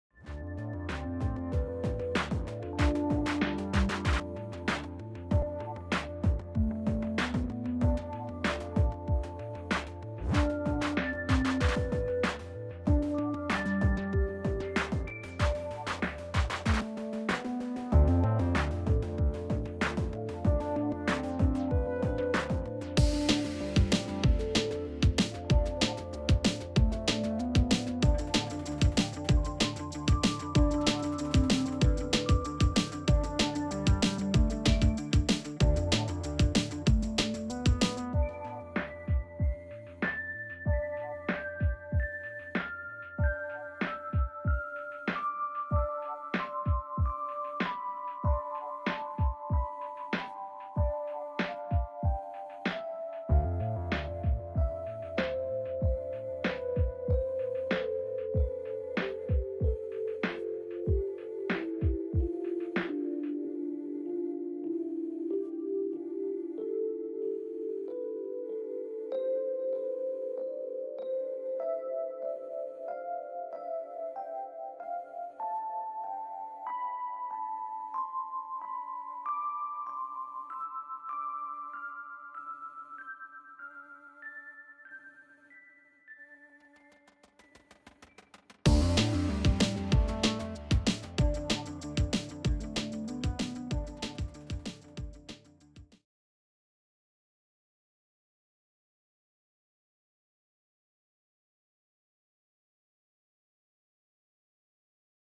Aufgemacht: kinder bueno
Hier aufgemacht und angeschnitten - die Riegelchen von kinder bueno.